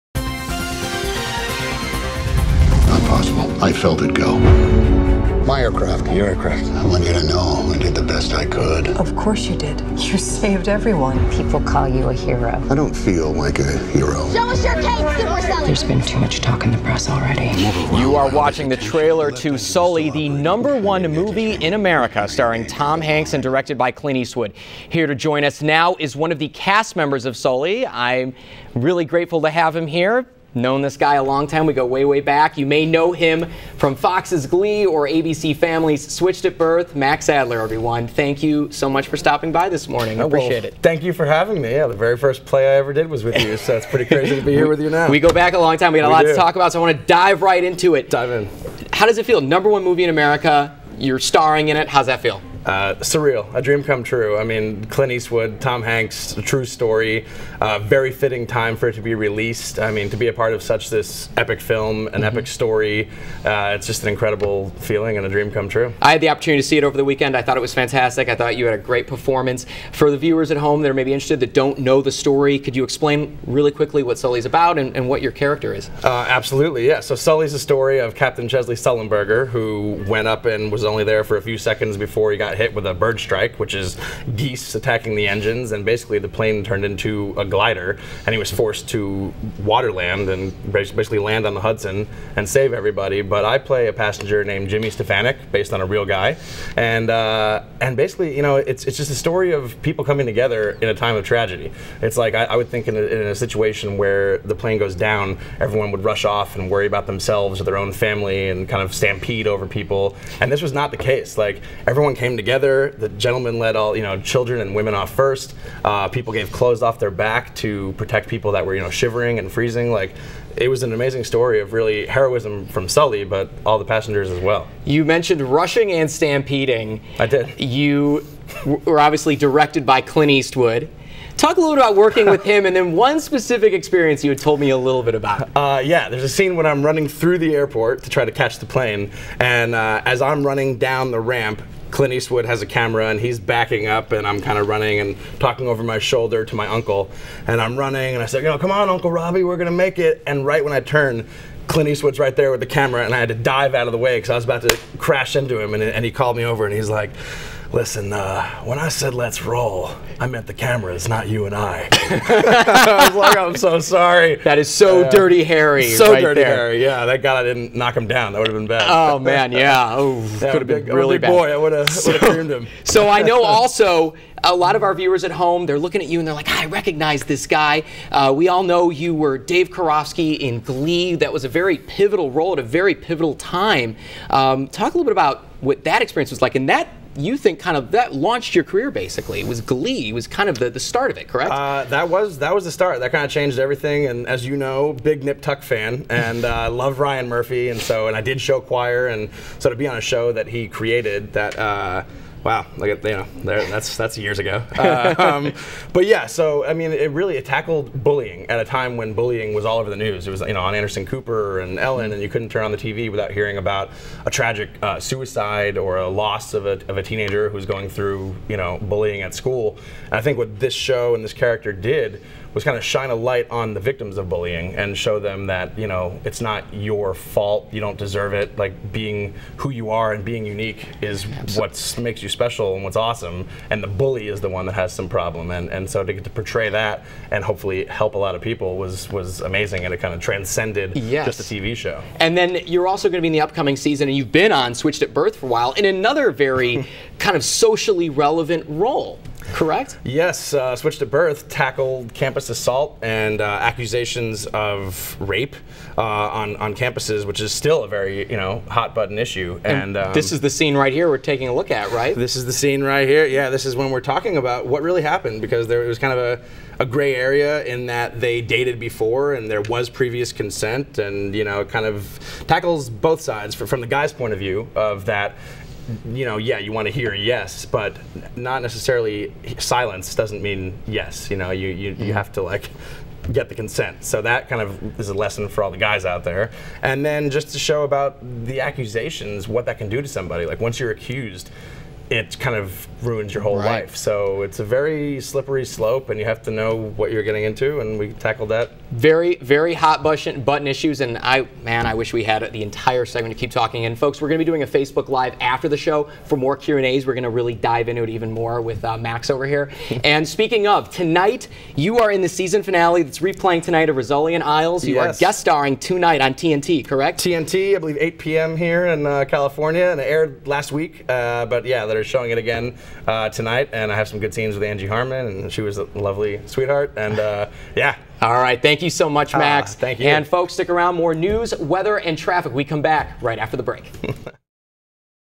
Max Adler on KKFX Fox-11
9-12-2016 Max Adler joined our show to talk about his work on "SULLY" (in theaters now), "GLEE", and "SWITCHED AT BIRTH"